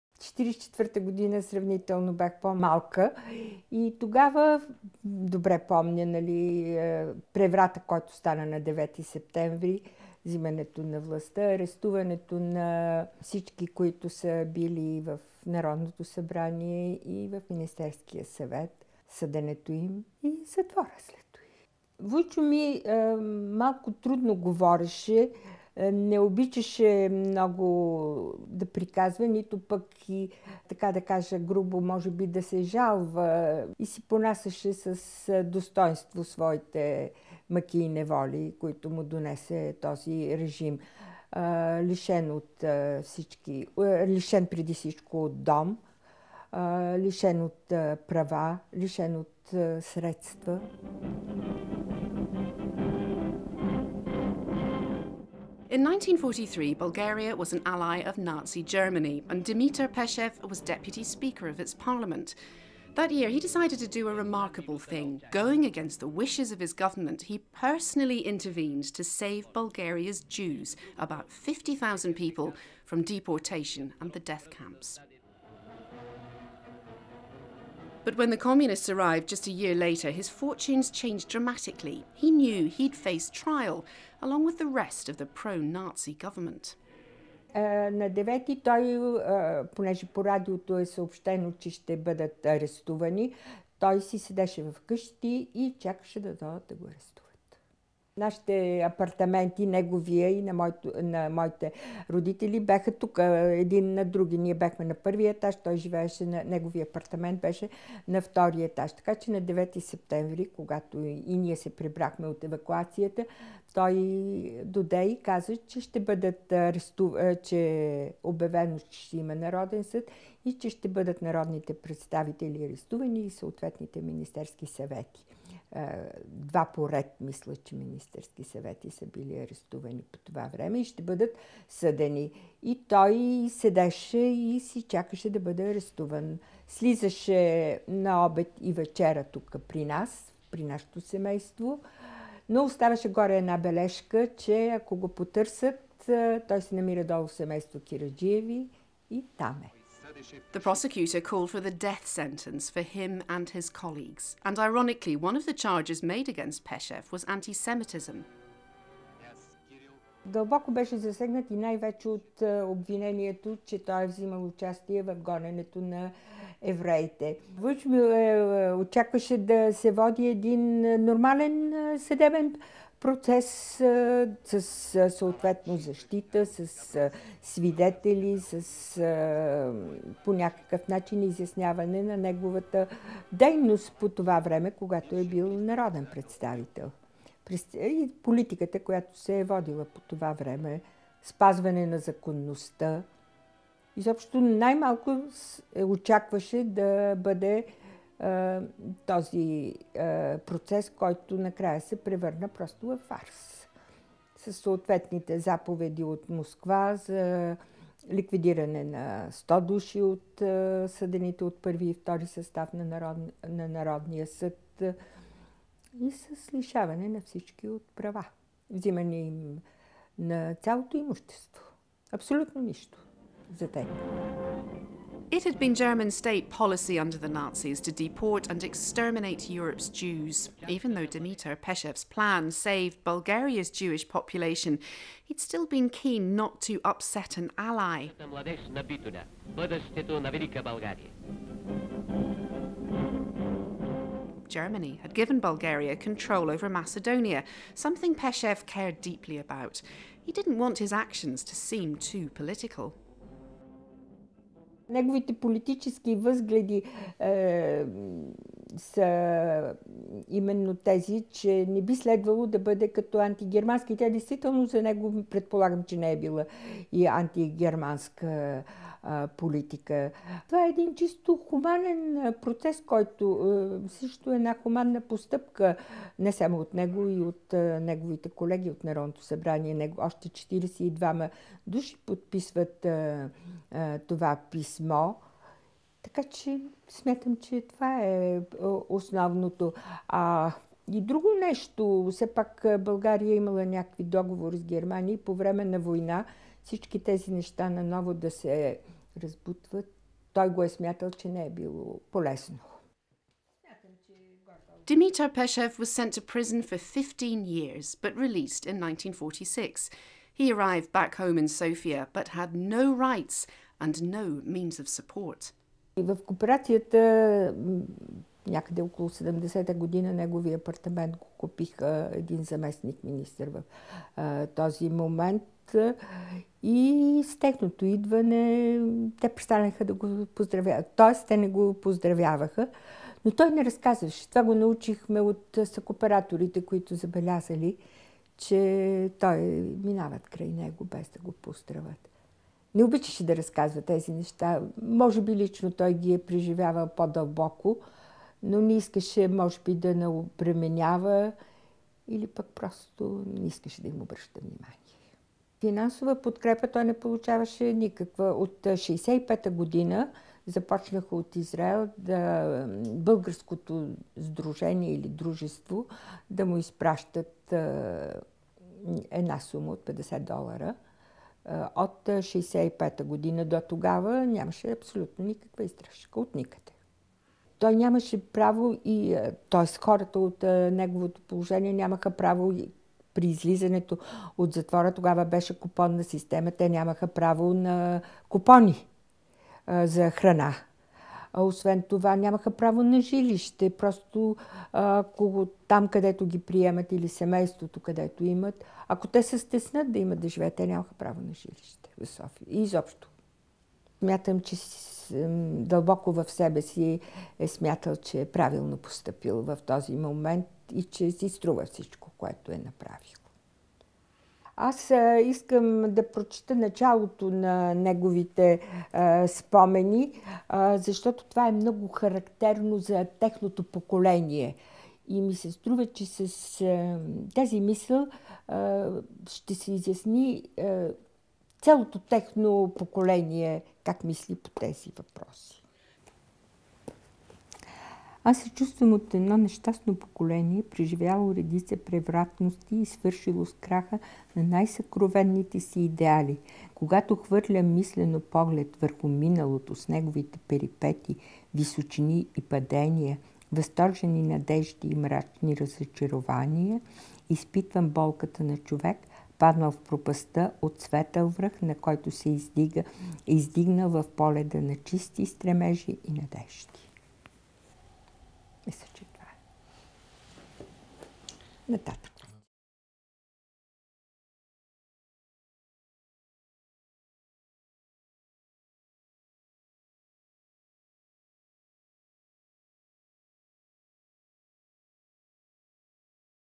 My Uncle, Bulgaria's Schindler
73 years ago on 10 March 1943 Bulgaria's pro-Nazi government decided to defy Berlin and halt the deportation of Bulgaria's 50.000 Jews. This was down to the actions of one man - Dimitar Peshev. Just two years later he faced Communist justice and found himself on trial for his life.

His niece Kaluda Kiradjieva remembers.